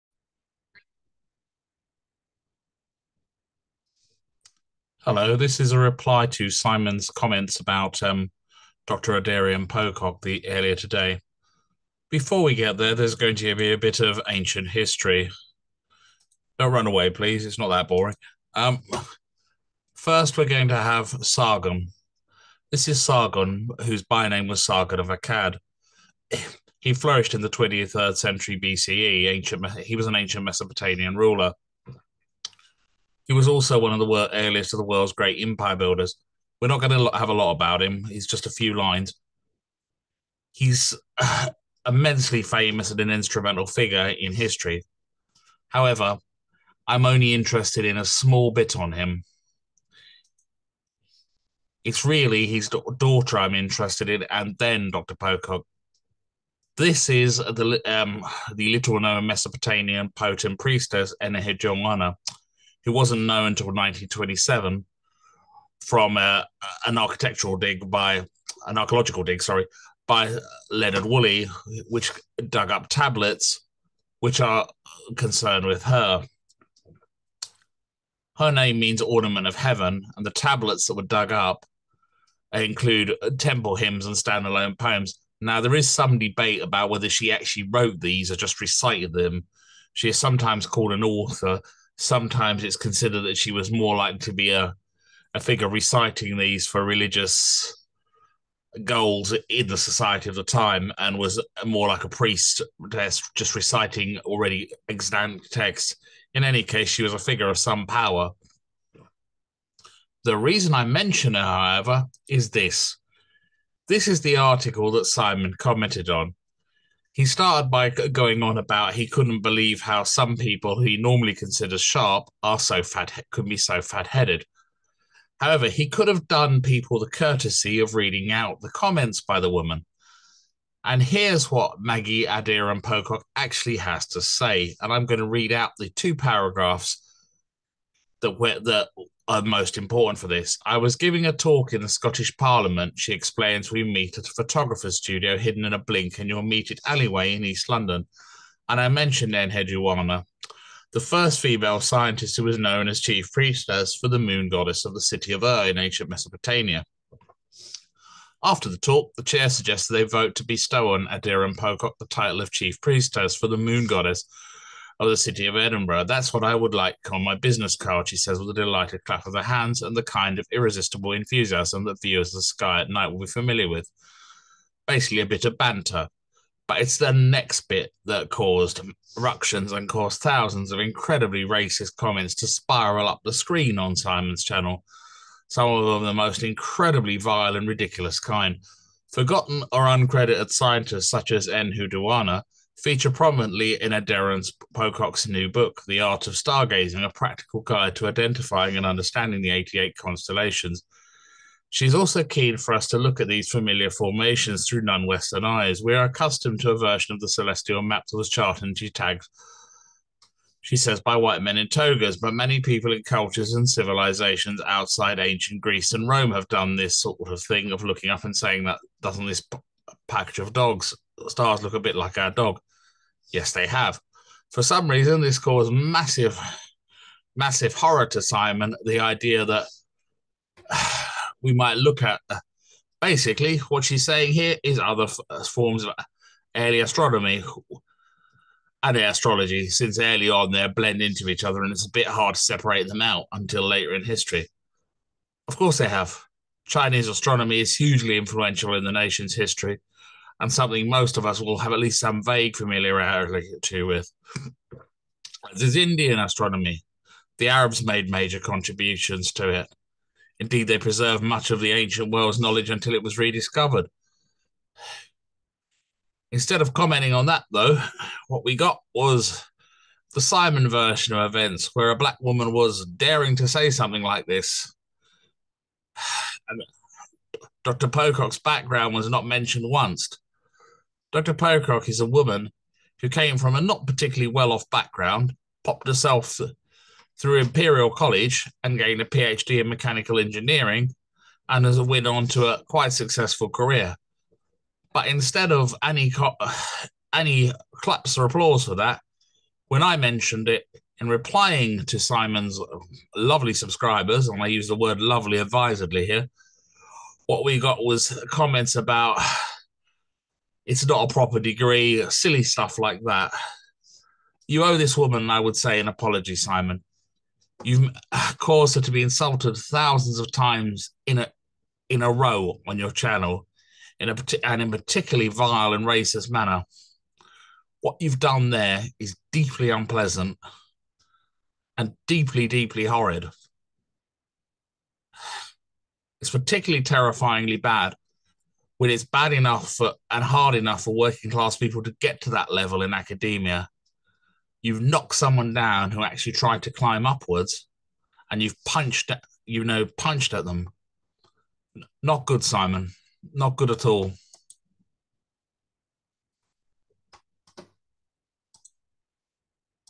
A Reply to History Debunked Regarding Dr. Maggie Aderin-Pocock.
This is a reply to Simon's video about Dr. Maggie Aderin-Pocock which I feel misleadingly casts her in a poor light and caused a huge number of racist remarks to be made about her: